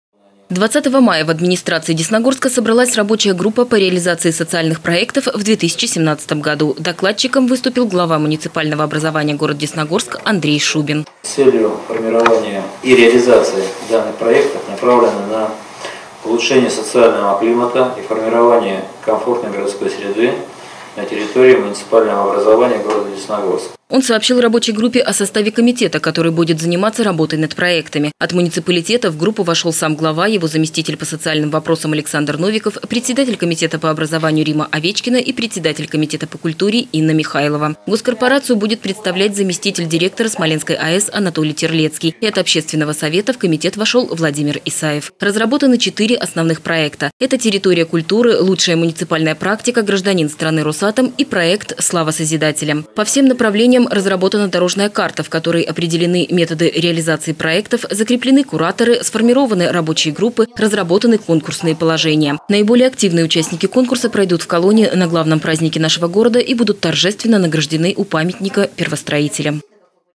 Десна-ТВ: Реализация проектов Госкорпорации-2017 стартовала
Уже не первый год в городах расположения объектов атомной отрасли реализуются совместные социальные проекты Госкорпорации «Росатом» и муниципалитетов этих районов.